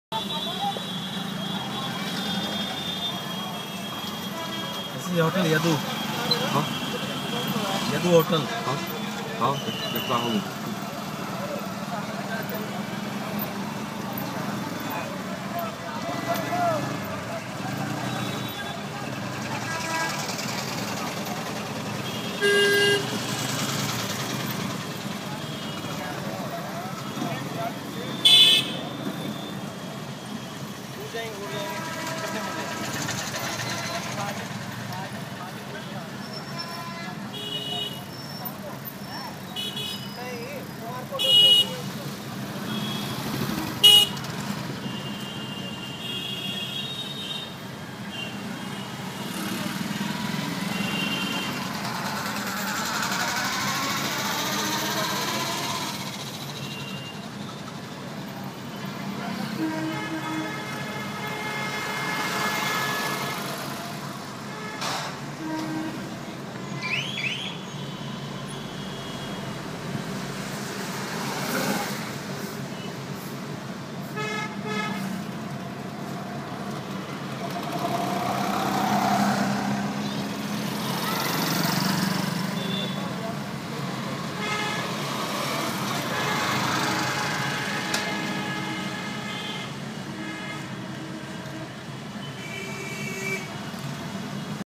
Indian traffic, part 1 of 2
Traffic in India, view from the car, sitting beside the driver